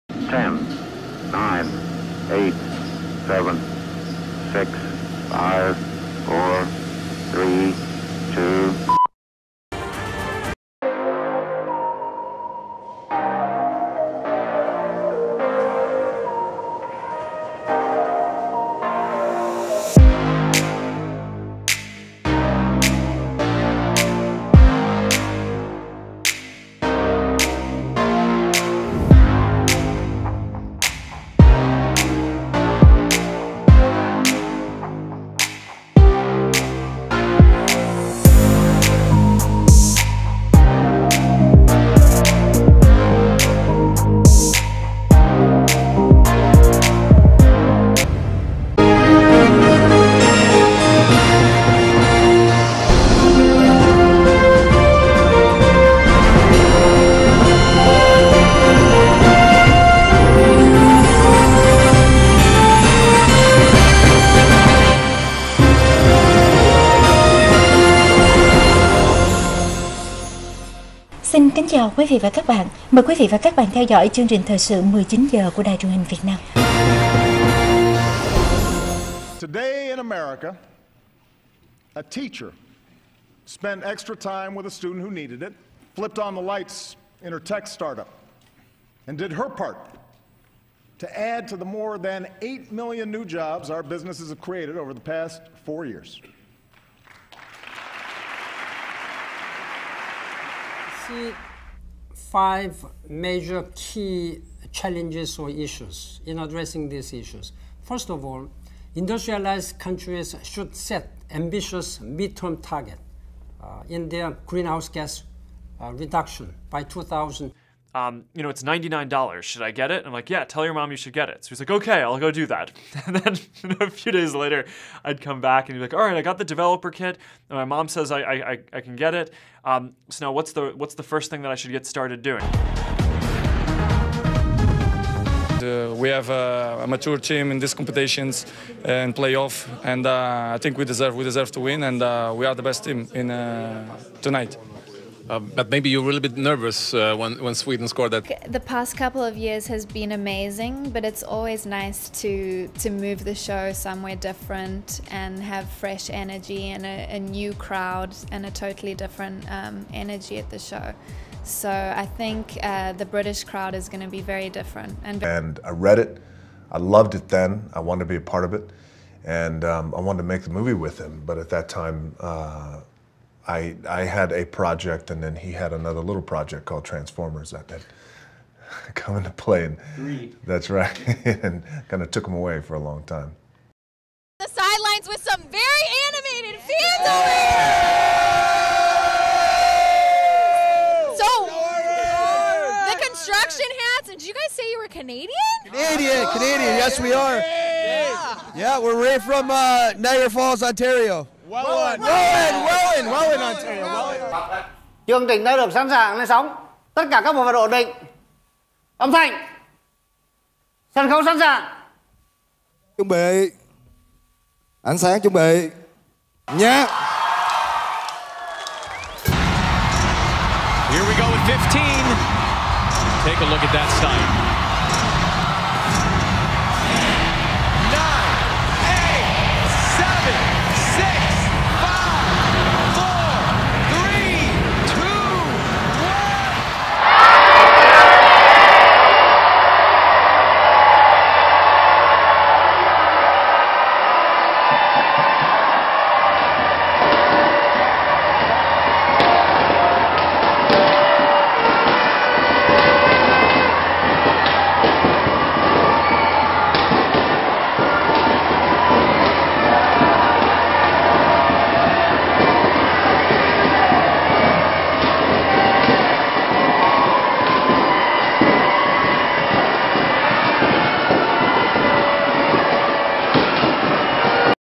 [Parody] Bản tin thời sự Hài hước cực kỳ bá đạo - mừng sinh nhật công ty Roselle
Nhân dịp sinh nhật công ty tròn 2 tuổi, tôi & các đồng nghiệp đã thực hiện 1 clip để phát trên ngày sinh nhật.
Clip là cái nhìn hài hước từ Parady bản tin thời sự & các nhân vật nổi tiếng thế giới. Mục đích là funny & để lại kỉ niệm cho các thành viên của công ty thôi qua Parady Bản tin thời sự